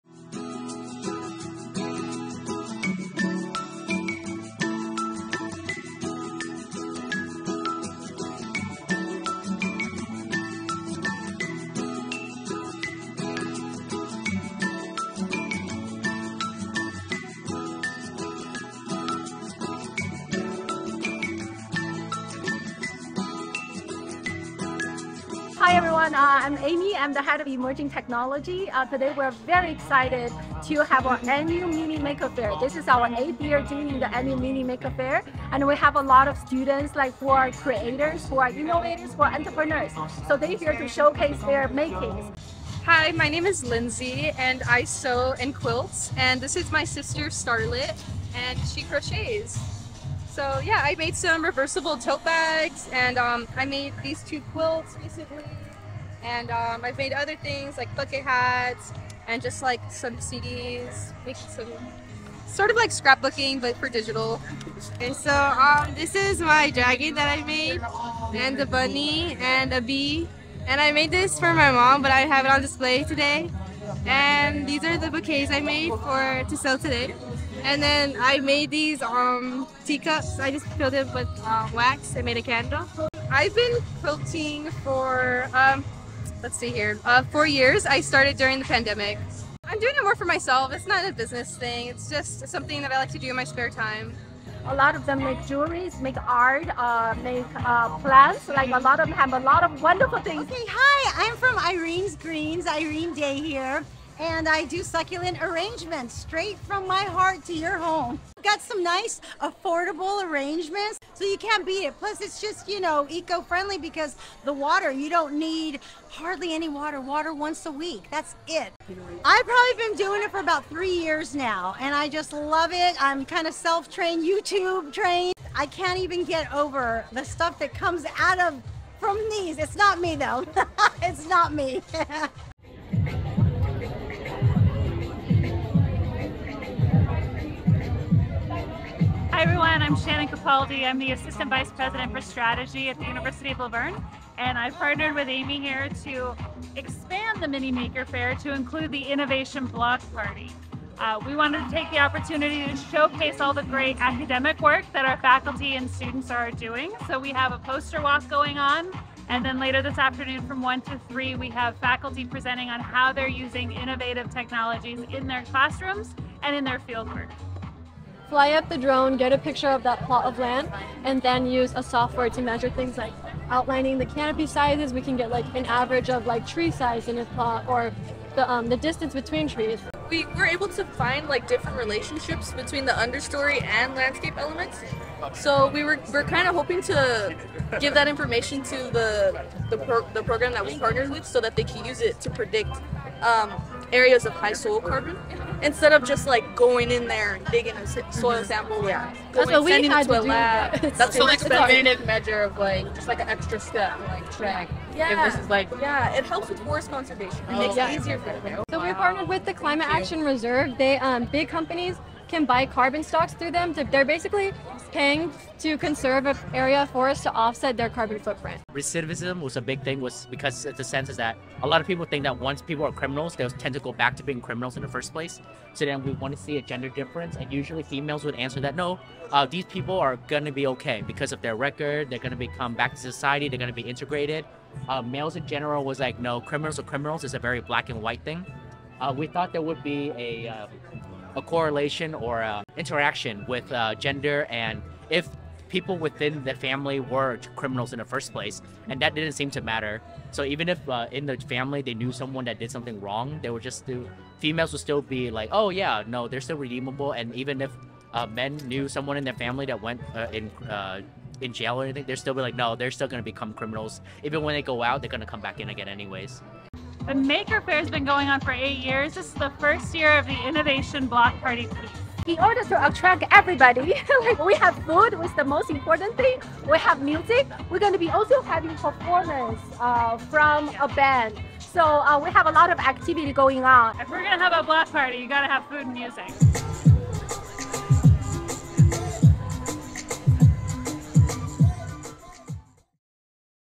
University of La Verne - Mini Maker Fair and Innovation Block Party 24'
The Mini Maker Fair and Innovation Block Party on May 1 at the University of La Verne was an exciting celebration of the creativity and innovation of our students and faculty. Students enjoyed the festivities by delving into a diverse range of exhibits, posters, and presentations highlighting the captivating scholarly endeavors of both students and faculty.

From 11:30 am to 1:00 pm on Citrus Lawn, the Mini Maker Fair had a wide range of exhibits with a delightful selection of handmade items for purchase. Additionally, a poster walk of students’ work was on display to showcase the interesting and impressive work our students completed. Students were also able to indulge in a special lunch while enjoying the beats of DJ Santana.